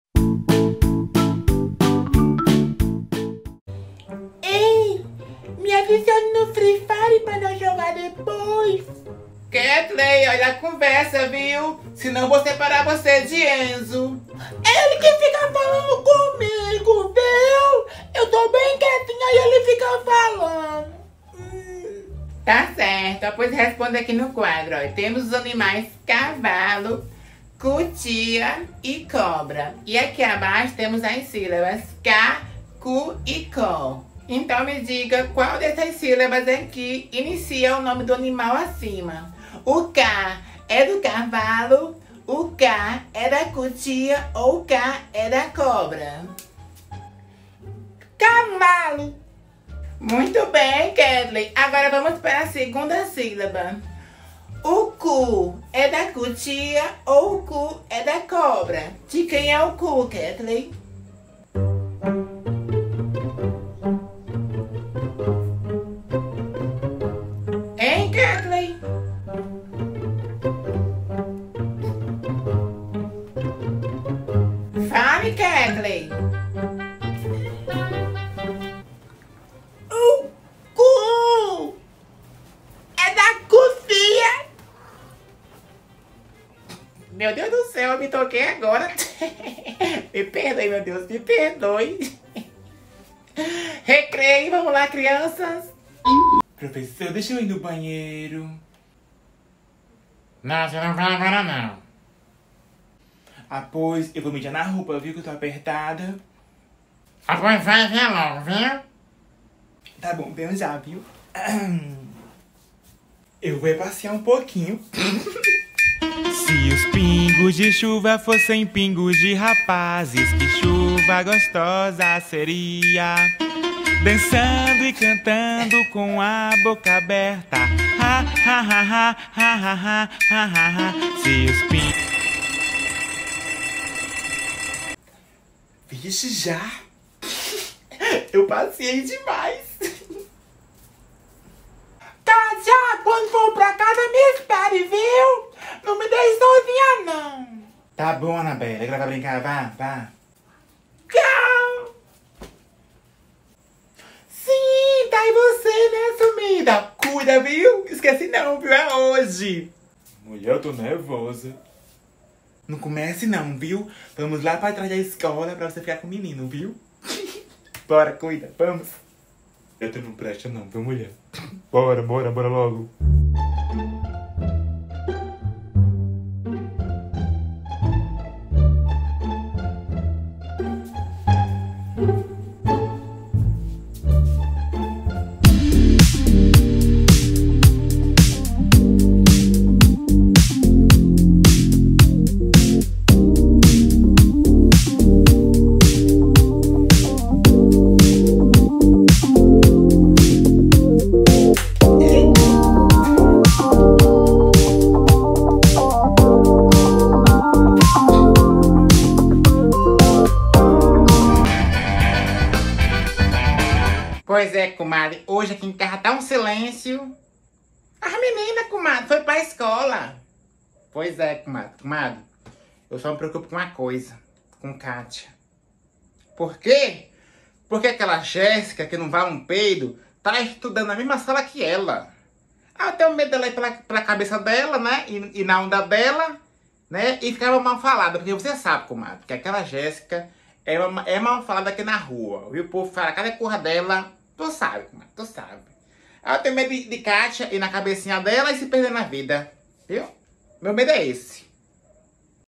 Primeiro dia de aula de Ketley e Katia na escola - volta as aulas
volta as aulas 2021. primeiro dia na escola. Ketley na escola, katia na escola
katia indo mais jessica pra de tras da escola pra ficar com um menino